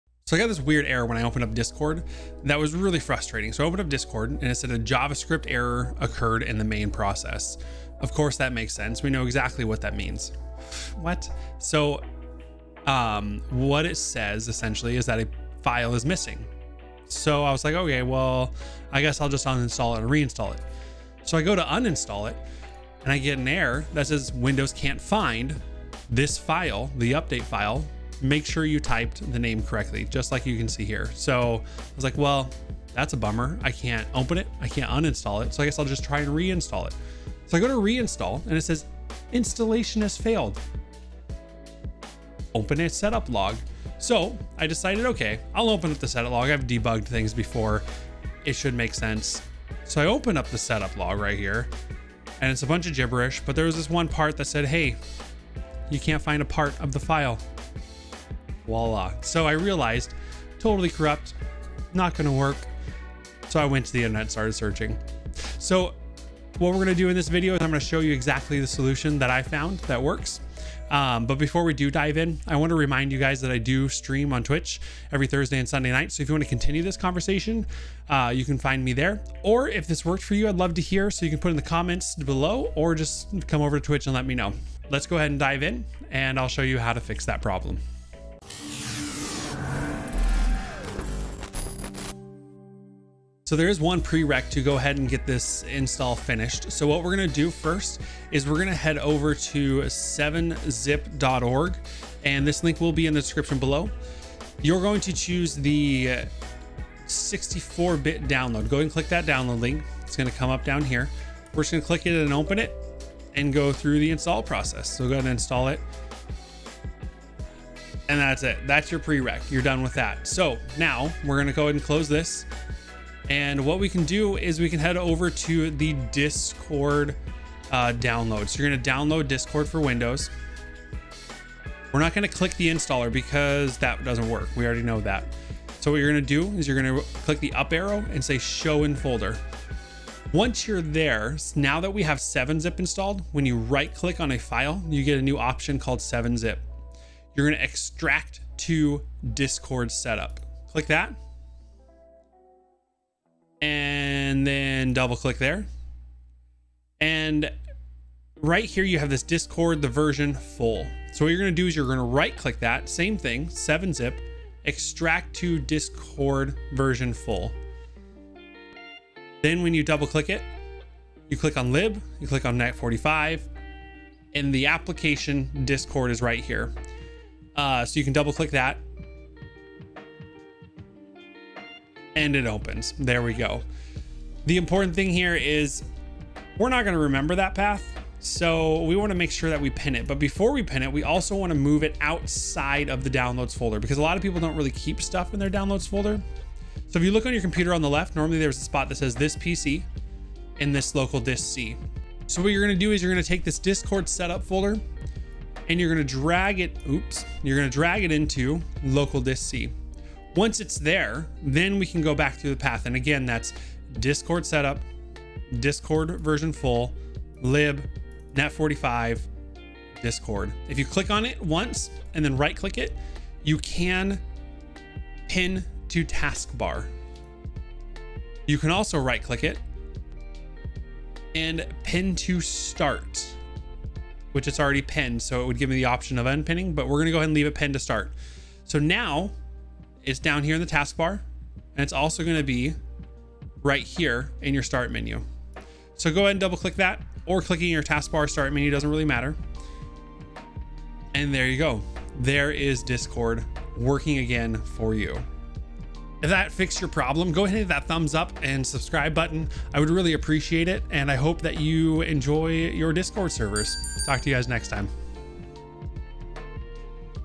Fix | Discord Won't Open | A JavaScript error occurred in the main process
I got a strange error in discord "A JavaScript error occurred in the main process" I could not install, uninstall or do anything. So I figured out the fix for this error and here it is. I hope this helps you!